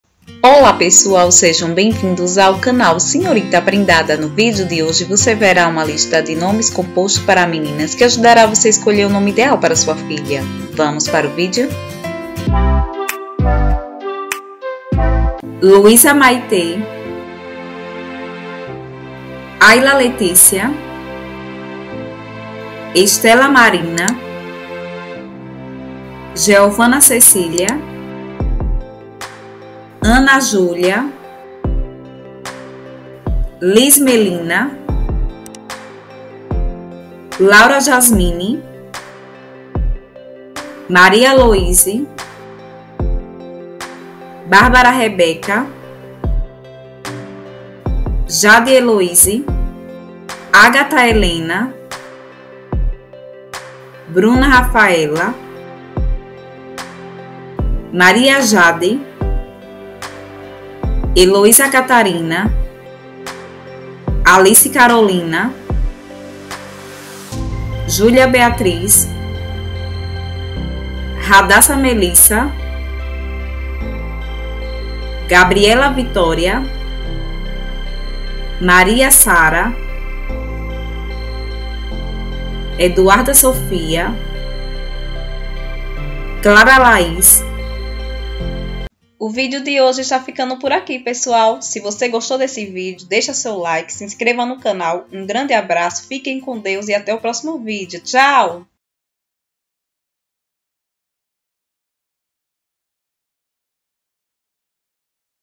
NOMES COMPOSTOS FEMININOS
Nesse vídeo você encontrará uma lista de opções de lindos compostos para femininos que ajudará a você a escolher o nome ideal para a sua filha.